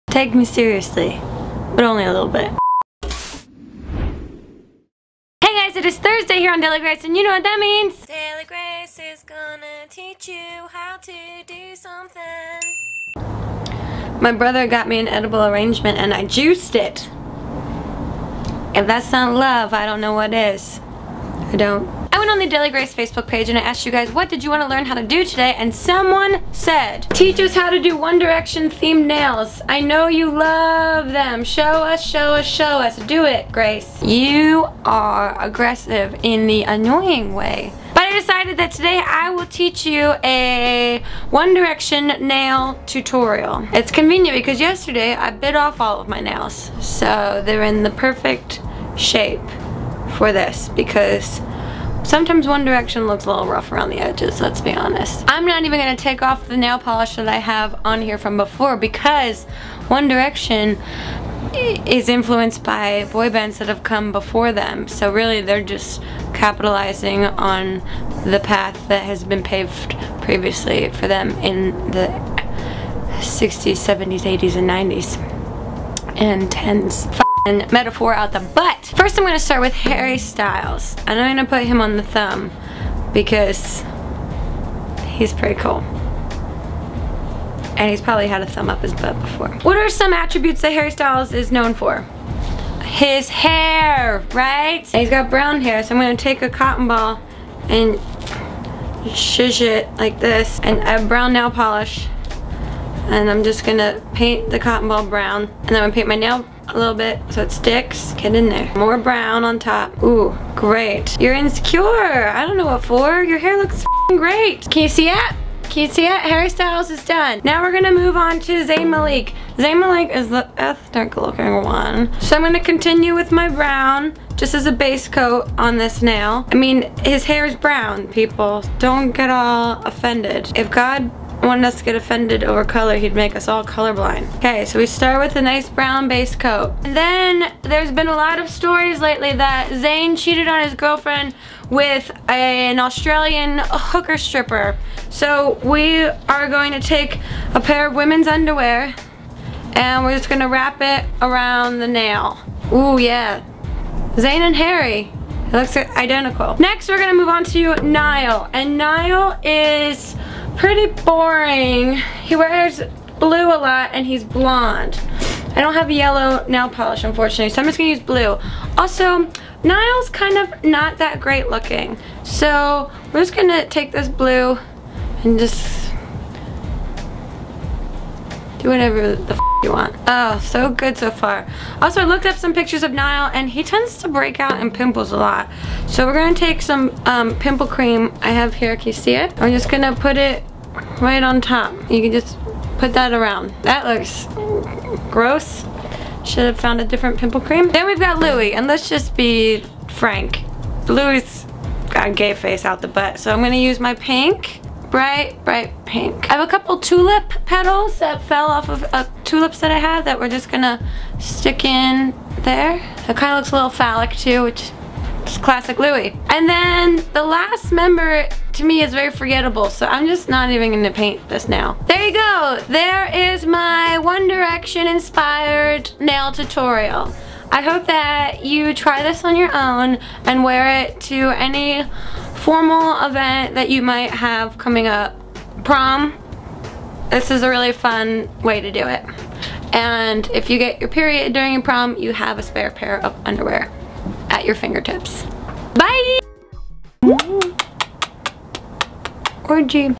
ONE DIRECTION NAIL TUTORIAL
DailyGrace gives you her ONE DIRECTION nail tutorial.